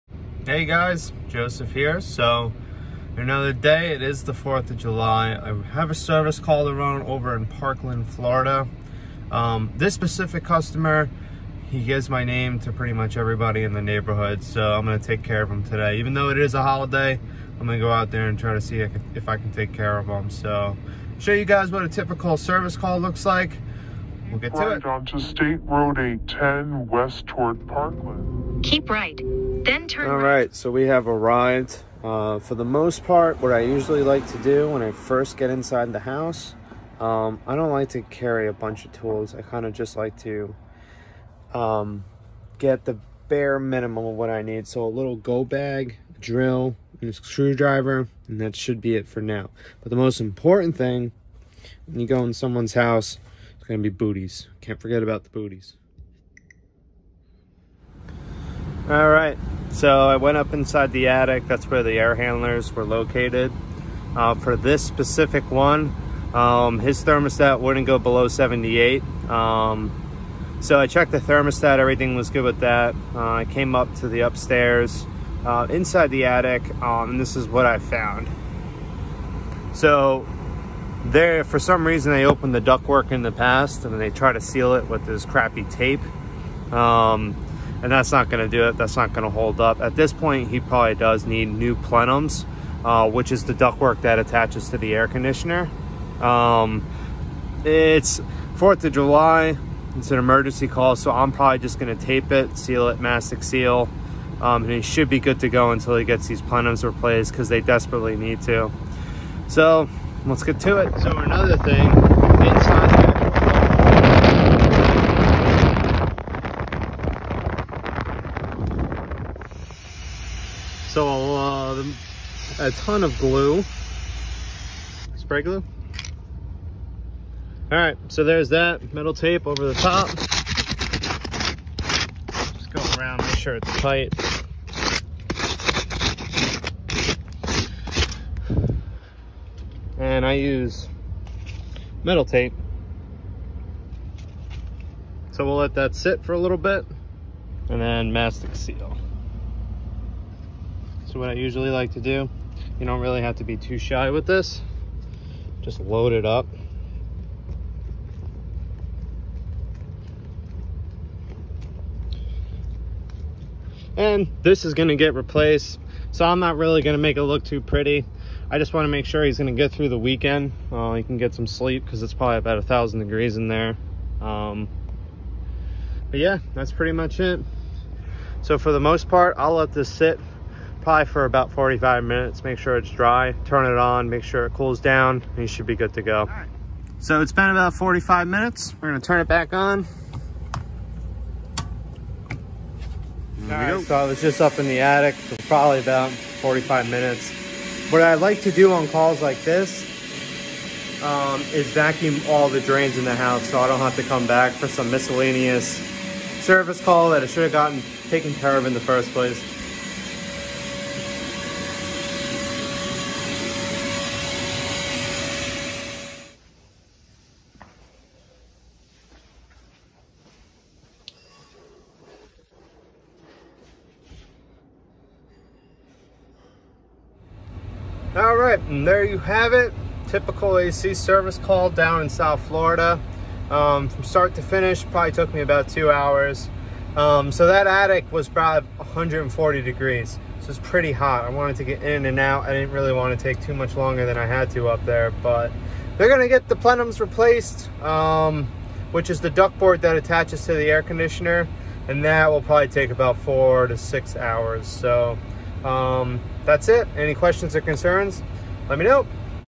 Air Conditioning Service Call, Fix Duct Work, Duct Work Repair. Typical South Florida Service Call
Ac repair and Service!